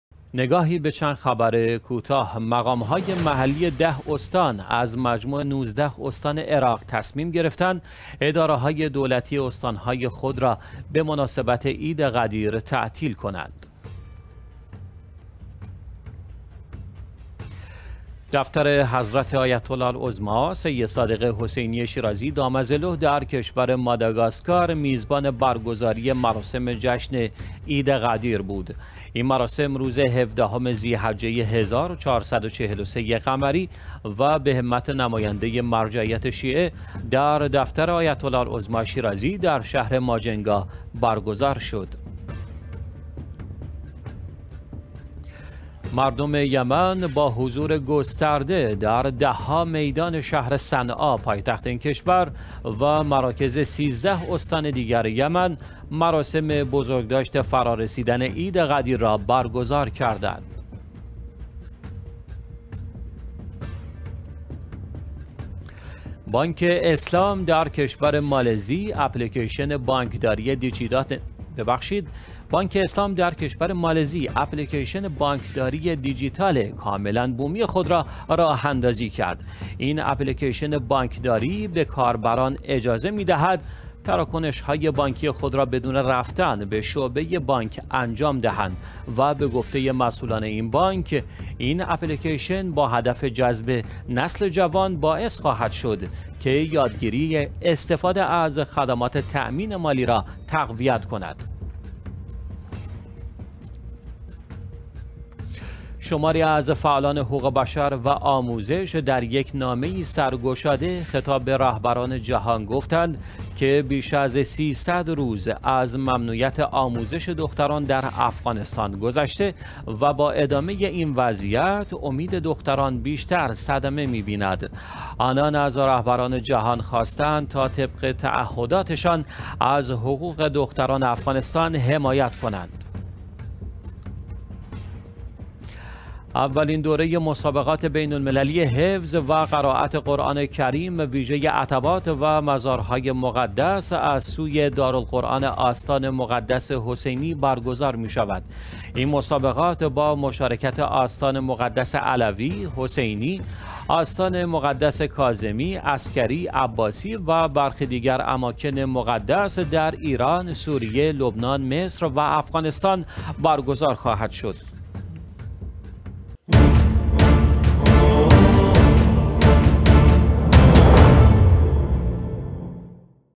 نگاهی به چند خبر کوتاه: دوشنبه 27 تير ماه 1401
۱)تعطیلی اداره های دولتی 10 استان عراق به مناسبت عید غدیر
۲)برگزارى جشن عيد غدير در ماداگاسكار
۳)حضور گسترده مردم یمن در مراسم بزرگداشت عید غدیر
۴)راه‌اندازی اپلیکیشن بومی «بانک اسلام» در مالزی
۵)نامه فعالان حقوق بشر به رهبران جهان برای تحصیل دختران در افغانستان
۶)برگزاری مسابقات‌ بین‌المللی قرآن توسط آستان مقدس حسینی علیه السلام